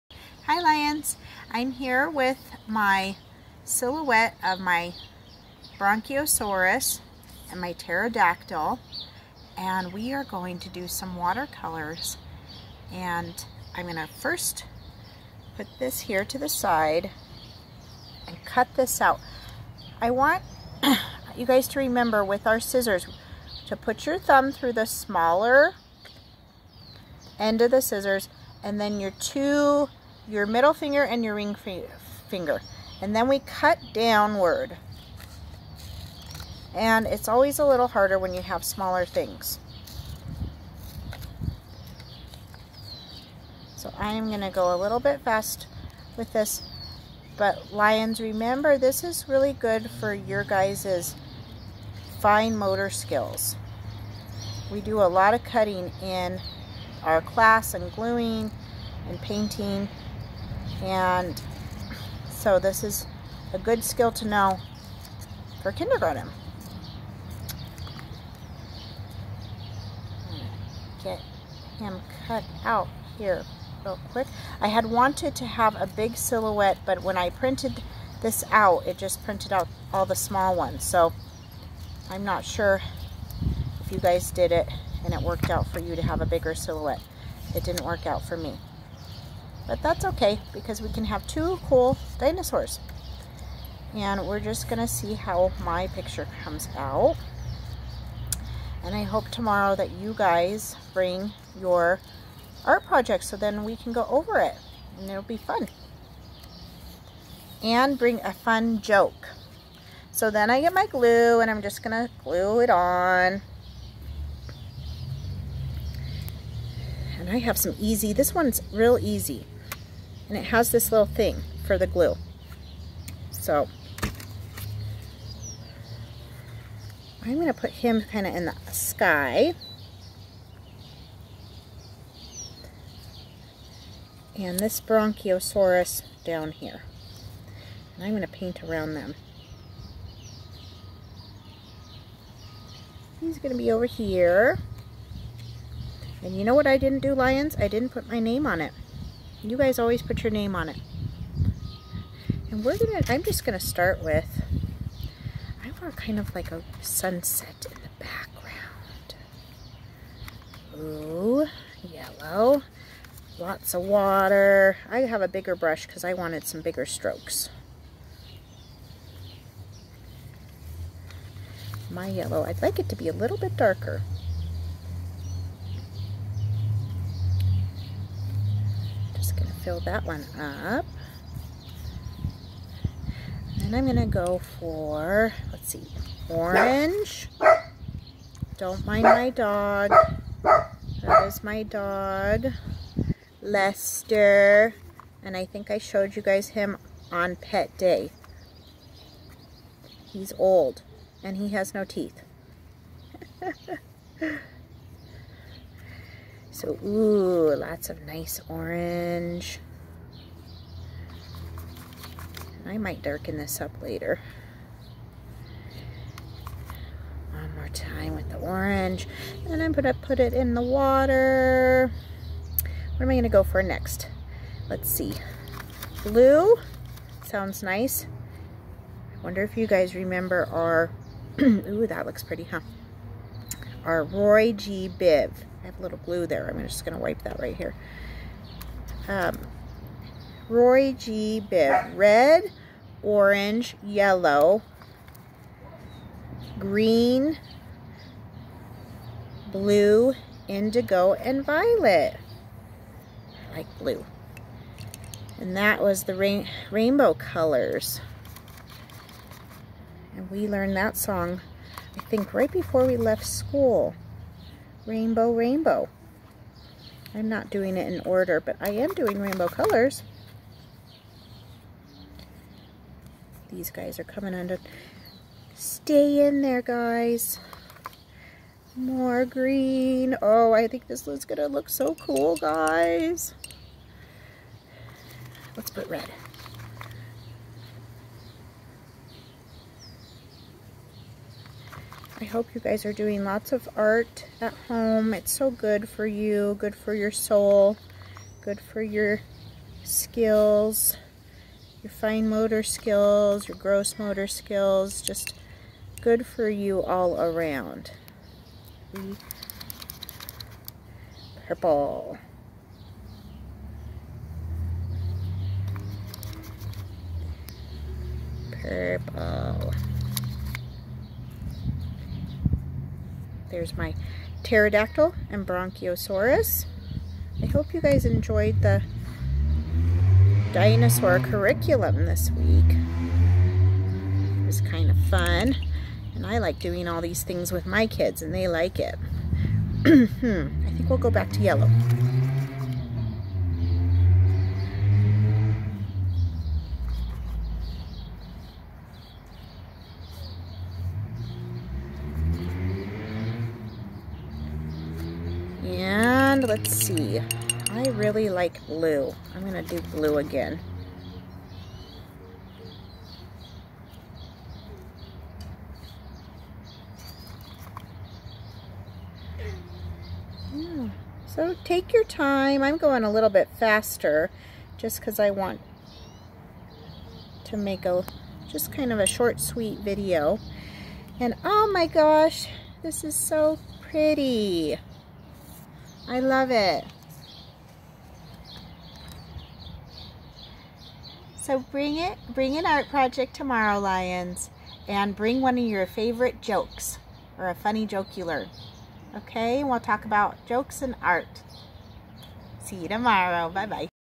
Creative Art!🎨🖼
Miss Monica makes dinosaur silhouettes and water color!